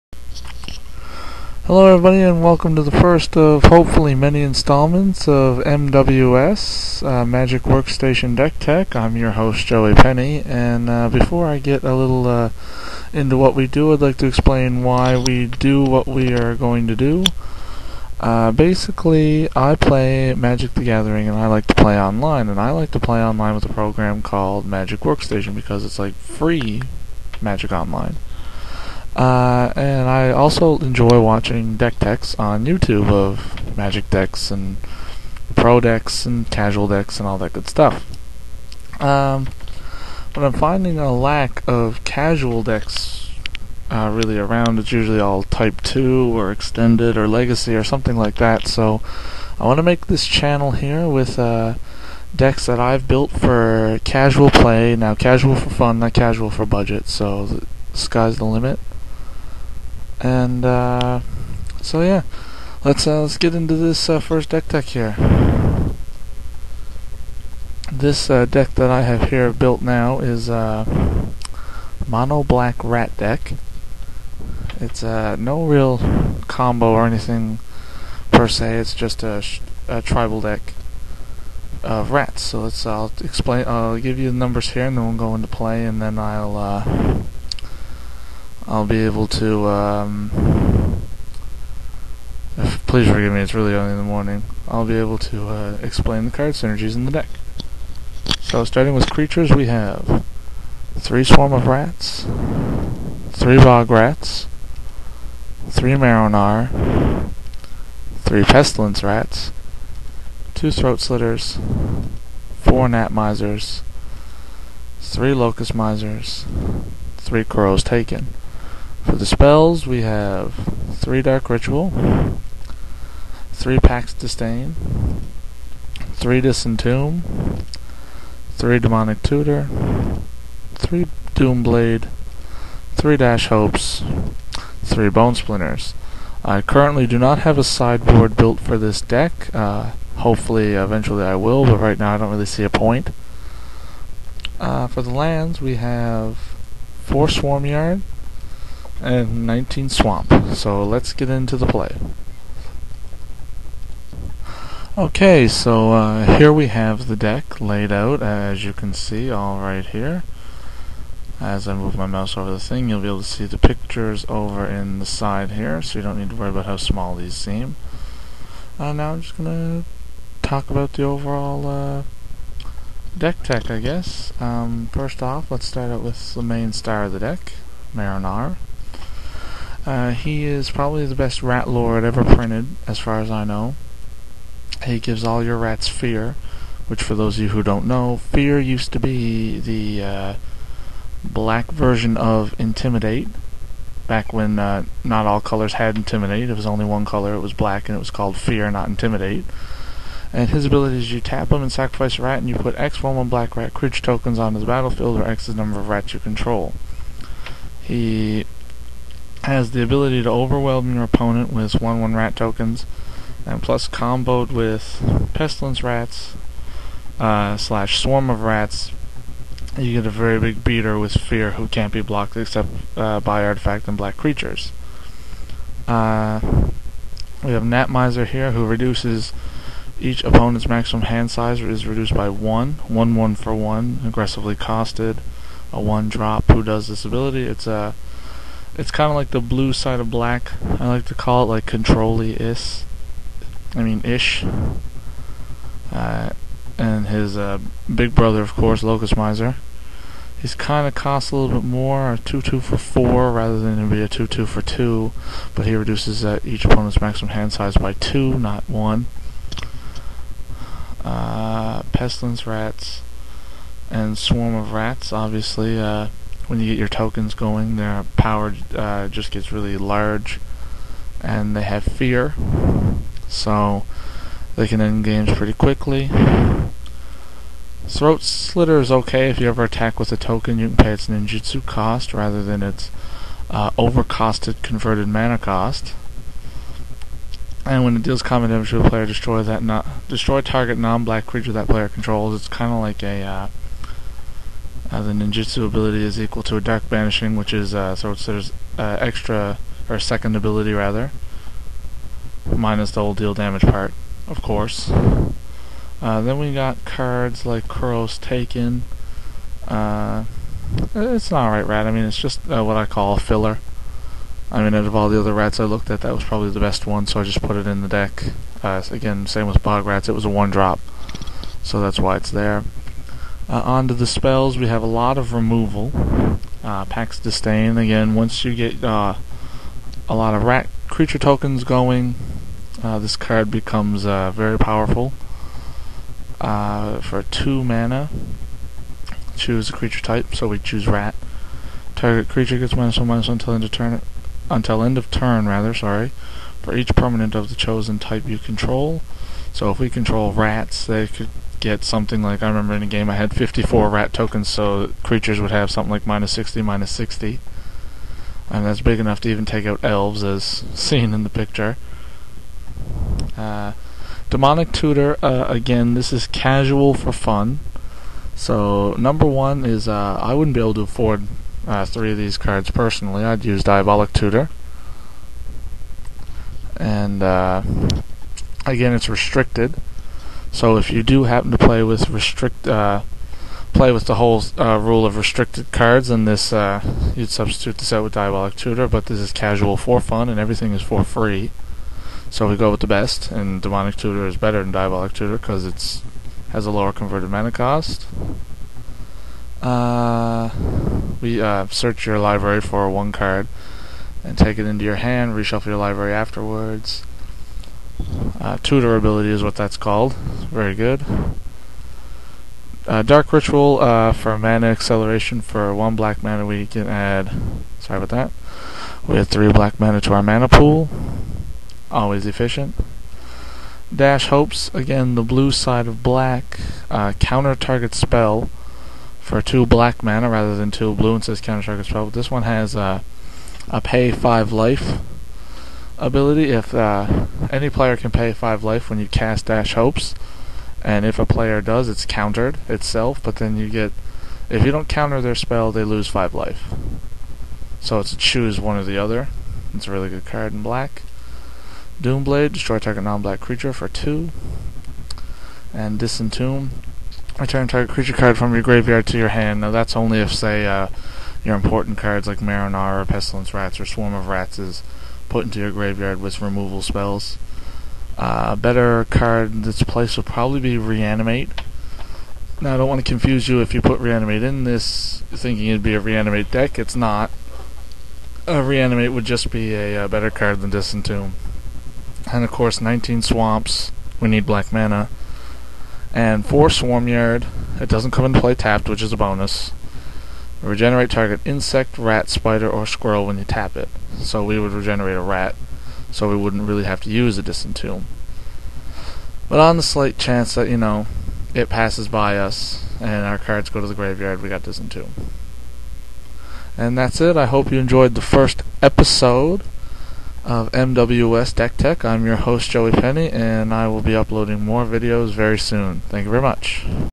MWS Deck Tech 1: Mono Black Rats
Hello everyone! Today i go over a deck i built a little while ago, mono black Rats! (p.s. sorry about the not so great audio, i don't have the best mic and i didn't want to wake anyone)